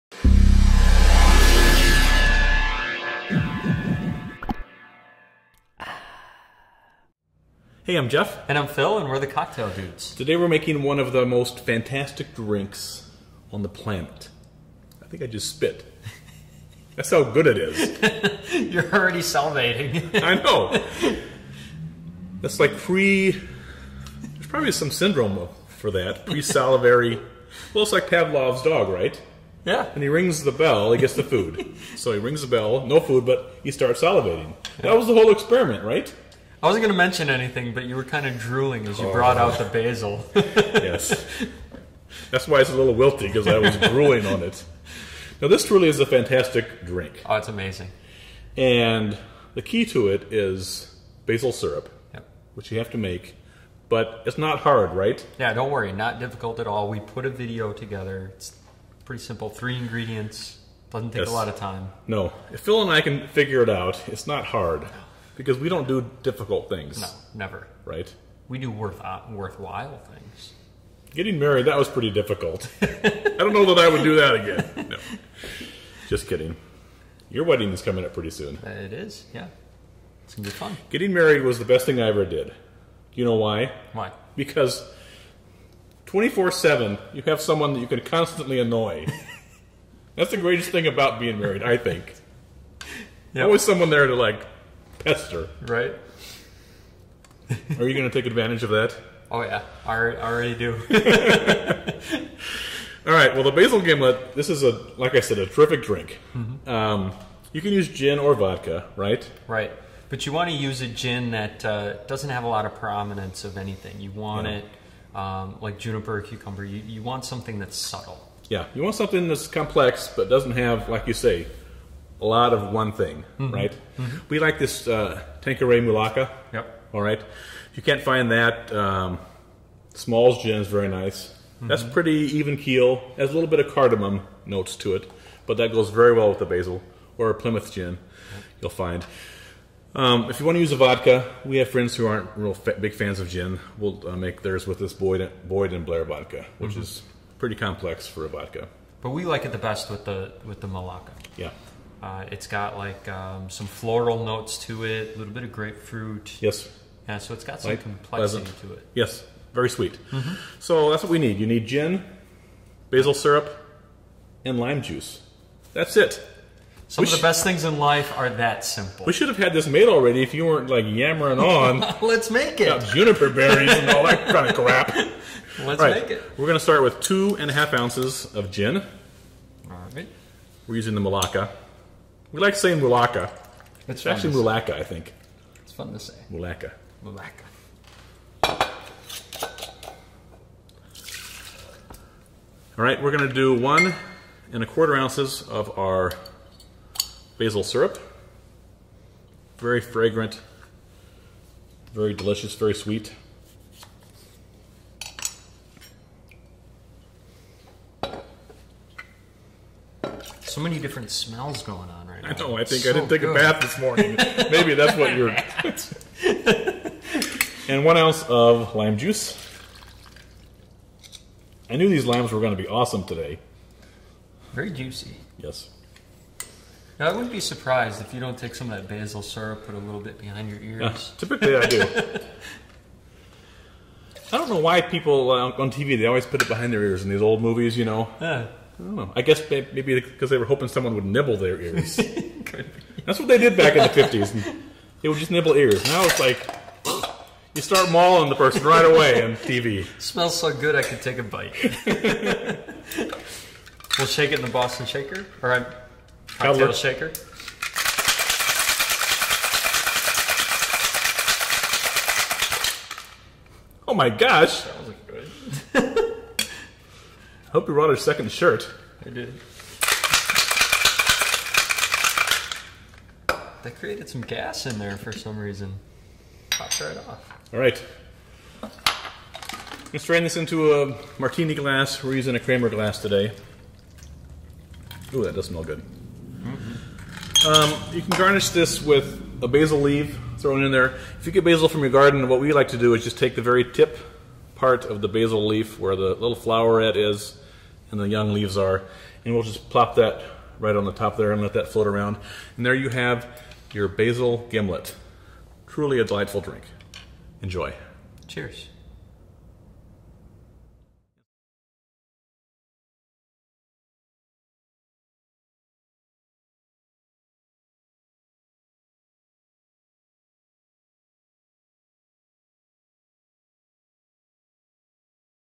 Basil Gimlet from the Cocktail Dudes
This is one of the most fantastic drinks on the planet. The mere mention of its name, in fact, gets Jeff drooling like a Pavlovian dog. "Ding!" We make use of basil syrup in this cocktail which really accentuates the botanicals of the gin and produces a crisp, delightful, and tasty drink. Cheers!